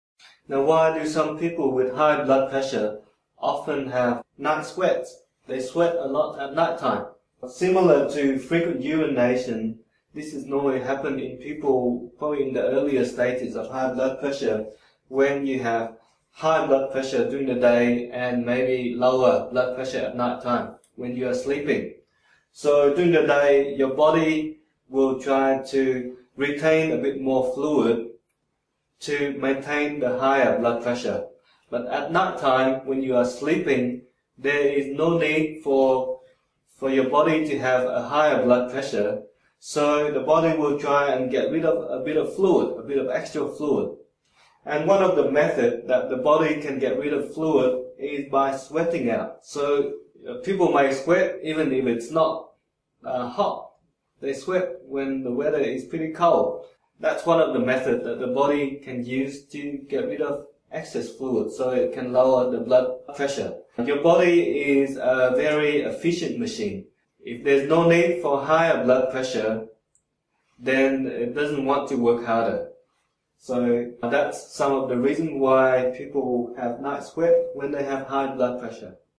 HBP 054- How is High Blood Pressure linked to night sweats
Discover how High Blood Pressure leads to night sweat.

Please also see our other videos on High Blood Pressure. 67 videos related to this topic has been uploaded on this channel.